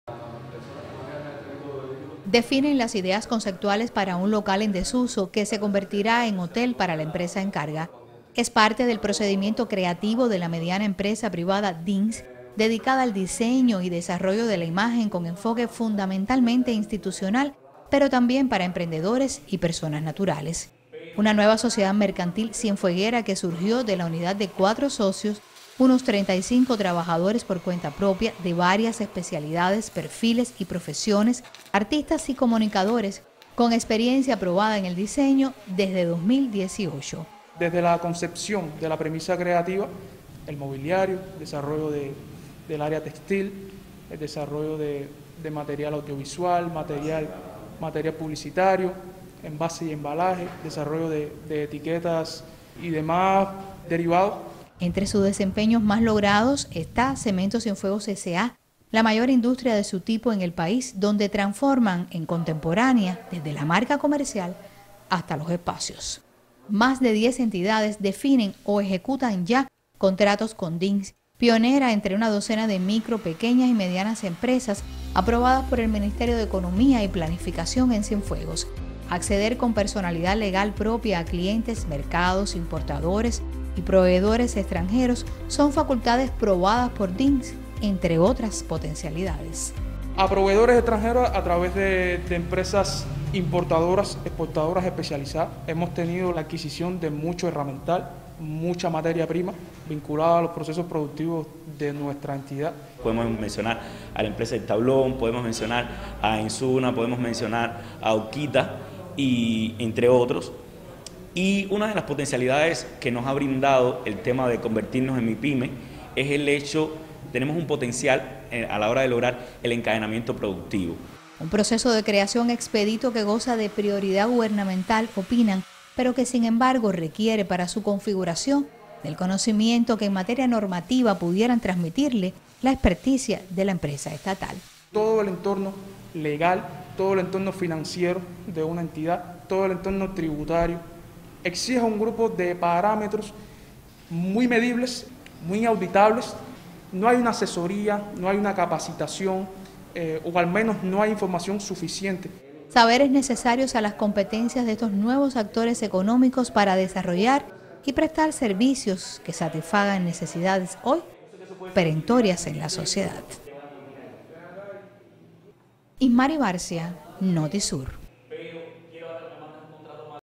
Expone MIPYME cienfueguera resultados de su desempeño
13 MIPYMES se han aprobado por el Ministerio de Economía y Planificación en Cienfuegos. DINS es pionera en su desempeño con unos de diez contratos con prestigiosas empresas del territorio, lo que afianza el desempeño de este nuevo actor económico.